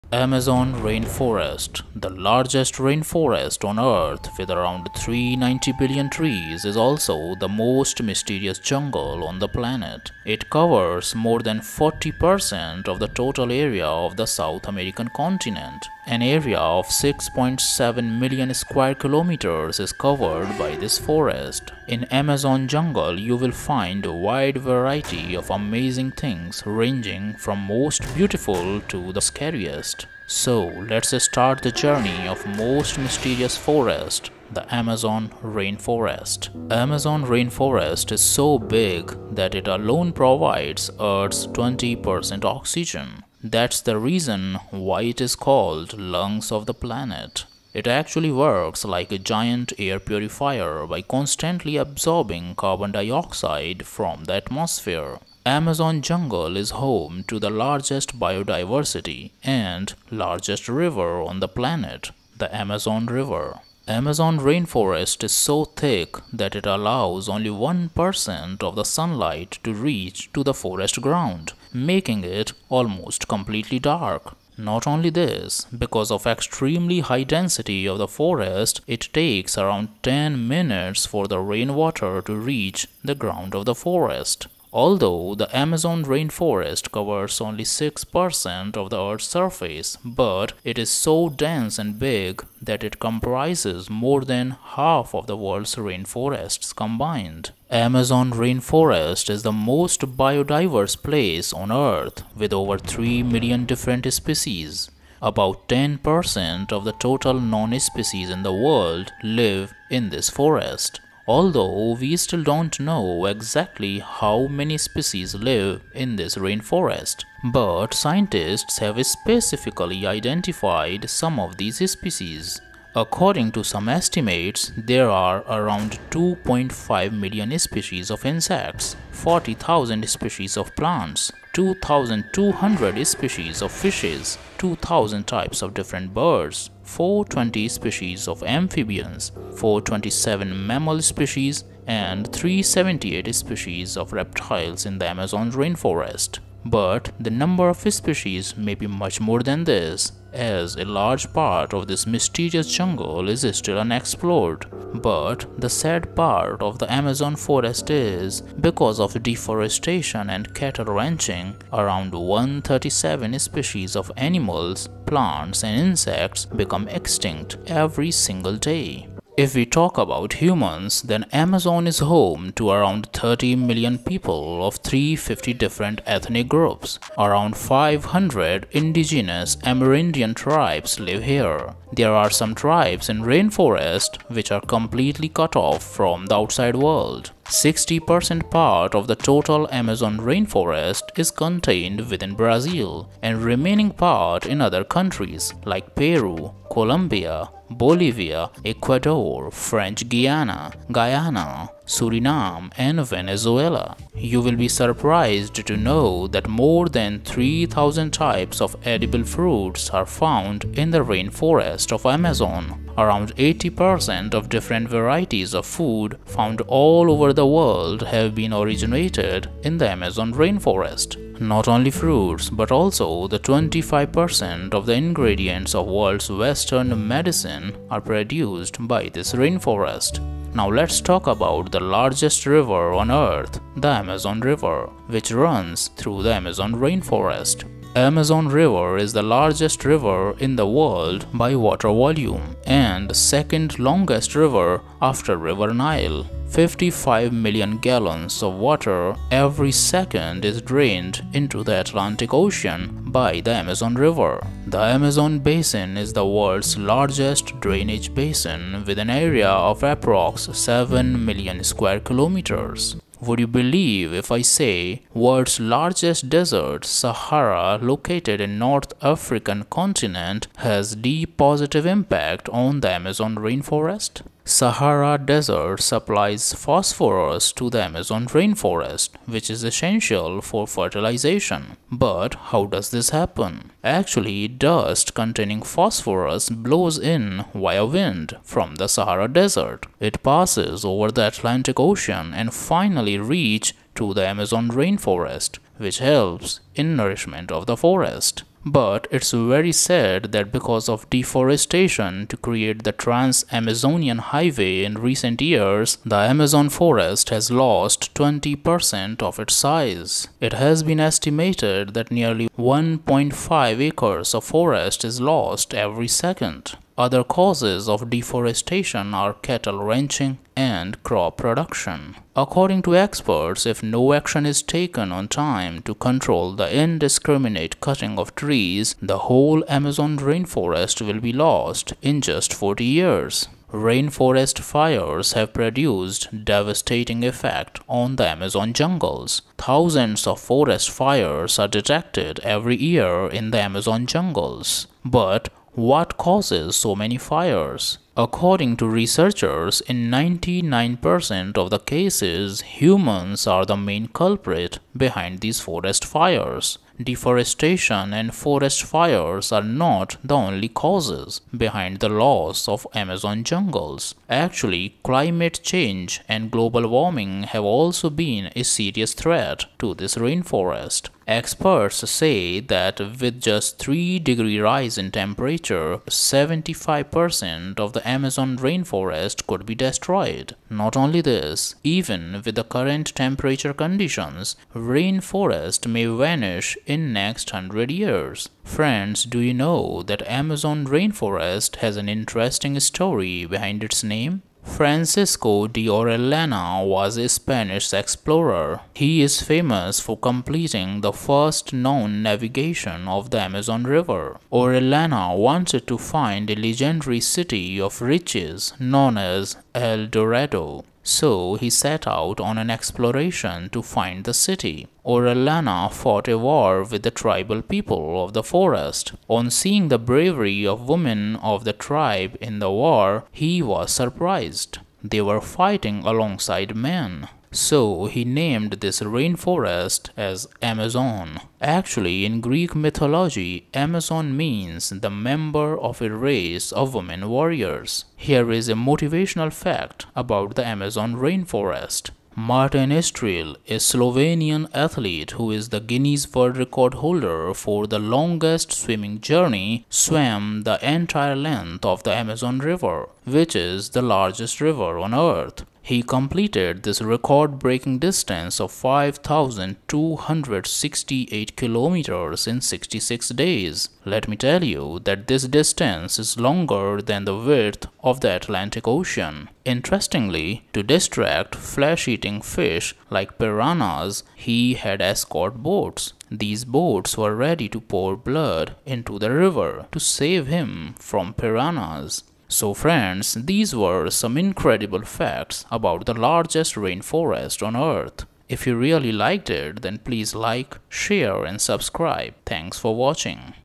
Amazon Forest Facts - Amazon Tribes - Amazon People - Amazon Tribe Women - Amazon Jungle
In this Amazon Rainforest Documentary you will know interesting facts about the Amazon rainforest (Amazon jungles). The video also covers Amazon tribes, Amazon people and the Amazon tribe women.
Here you will know about Amazon rainforest geography and location. Amazon rainforest animals, birds and plants have also been covered here.
The video also discusses Amazon rainforest tribes (aboriginals) and how Amazon rainforest has reduced in size due to Amazon rainforest fires and deforestation. You will also find mysteries of Amazon jungle in this video.

Amazon Rainforest- The largest rainforest on earth with around 390 billion trees, is also the most mysterious jungle on the planet.
It covers more than 40℅ of the total area of the South American continent. An area of 6.7 million square kms is covered by this forest.

Amazon rainforest is so big that it alone provides earth's 20℅ oxygen. That's the reason, why it called "lungs of the planet".
It actually works like giant air purifier by constantly absorbing Carbon Dioxide from the atmosphere.

Amazon jungle is home to the largest biodiversity and the largest river on the planet, the Amazon River.

Not only this, because of extremely high density of the forest, it takes around 10 minutes for the rain water to reach the ground of the forest.

Although the Amazon rainforest covers only 6℅ of the Earth surface but it's so dense and big that it comprises more than half of the world’s rainforests combined.

Amazon rainforest is the most biodiverse place on earth with over 3 million different species.
About 10℅ of the total known species in the world live in this forest.

According to some estimates there are around 2.5 million species of insects, 40,000 species of plants, 2200 species of fishes, 2000 types of different birds, 428 species of amphibians, 427 mammals species and 378 species of reptiles in the Amazon rainforest.

But the sad part of the Amazon forest is- because of deforestation and cattle ranching, around 137 species of animals, plants and insects become extinct every single day.

If we talk about humans then Amazon is home to around 30 million people of 350 different ethnic groups. Around 500 indigenous Amerindian tribes live here.
There are some tribes in the rainforest which are completely cut off from the outside world!

60℅ part of the total Amazon rainforest is contained within Brazil and remaining part in other countries like Peru, Colombia, Bolivia, Ecuador, French Guiana, Guyana,  Suriname and Venezuela.

You will be surprised to know
that more than 3,000 types of edible fruits are found in the rainforest of Amazon.
Around 80℅ of different varieties of food found all over the world have been originated in the Amazon rainforest.

Not only fruits but also the 25℅ of the ingredients of world's western medicine are produced by this rainforest.

Amazon River is the largest river in the world by water volume and second longest river after river Nile.

55 million gallons of water every second is drained into the Atlantic Ocean by the Amazon River.
The Amazon basin is the world's largest drainage basin with an area of approx. 7 million square kms.

World's largest desert Sahara located in North African continent has deep positive impact on the Amazon rainforest!!!
Sahara desert supplies phosphorus to the Amazon rainforest which is essential for fertilization.

But it's very sad that because of deforestation to create the Trans-Amazonian Highway in recent years, the Amazon forest has lost 20℅ o
f its size.
It has been estimated that nearly 1.5 acres of forest is lost every second.
Other causes of deforestation are cattle ranching and crop production.

Rainforest fires have produced devastating effect on the Amazon jungles. Thousands of forest fires are detected every year in the Amazon jungles.
But what causes so many fires.
According to researchers, in 99℅ of the cases, humans are the main culprit behind these forest fires.

Climate change and global warming have also been a serious threat to this rainforest.
Experts say that with just 3 degree rise in temperature, 75℅ of the Amazon rainforest could be destroyed.

Amazon rainforest has an interesting story behind it's name?
Francisco de Orellana, was a Spanish explorer.
He is famous for completing the first known navigation of the Amazon River.
Orellana wanted to find a legendary city of riches known as El Dorado, so he set out on an exploration to find the city.
Orellana fought a war with tribal people of the forest.
On seeing bravery of women of the tribe in the war, he was surprised. They were fighting alongside men.
So he named this rainforest as Amazon.
Actually, in Greek mythology, Amazon means the member of a race of women warriors.

Martin Strel, a Slovenian athlete who is the Guinness World Record holder for the longest swimming journey.
He swam the entire length of the Amazon river which is the largest river on earth.